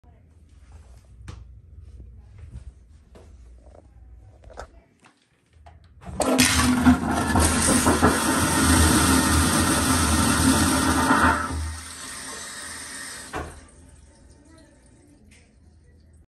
AcornVac toilet
Filmed at the pediatric specialties at Northwell Health Imaging at the Center for Advanced Medicine in New Hyde Park, NY